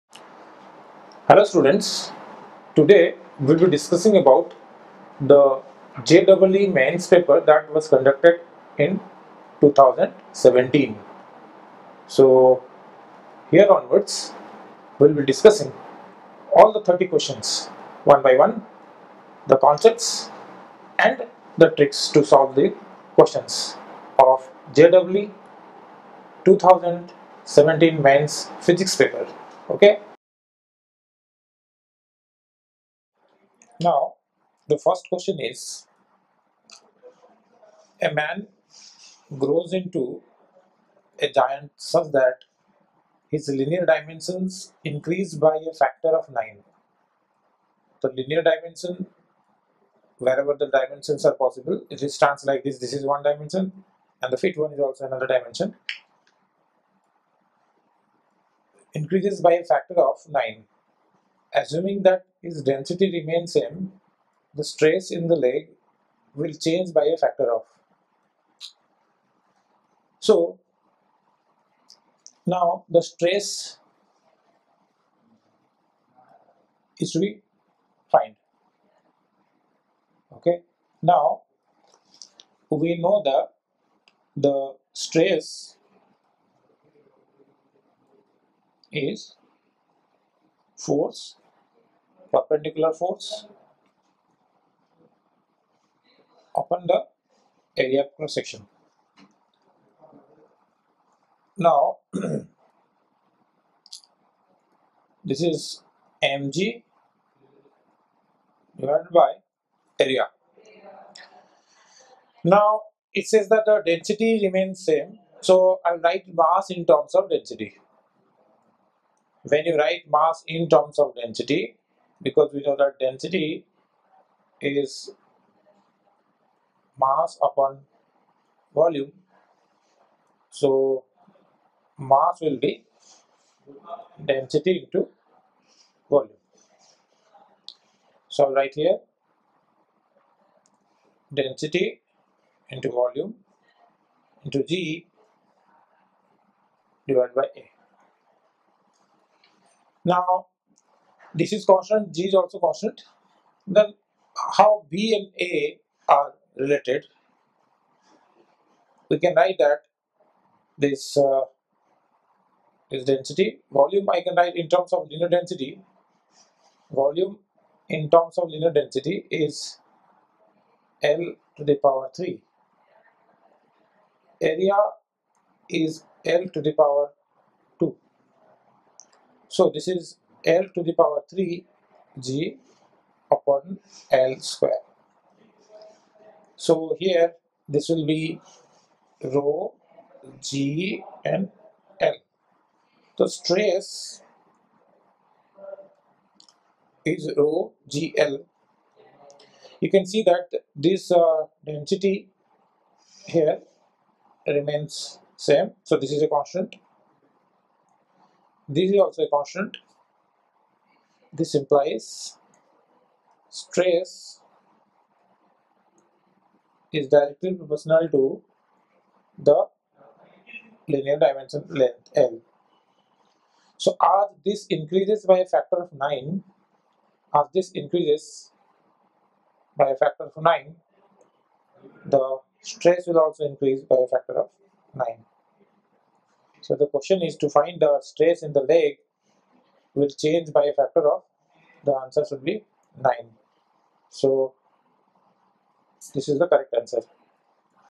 JEE Main 2017 Physics Solution - Question 1 - IIT-JEE Main Physics Paper Video Solution | Ekeeda.com
Video Lecture on Question 1 of JEE Main 2017 Physics Paper of Physics JEE Main 2017 Paper Video Solution.

Access the complete playlist of Physics - JEE Main 2017 Paper Solution

You can follow and Like us in following social media.

Happy Learning : )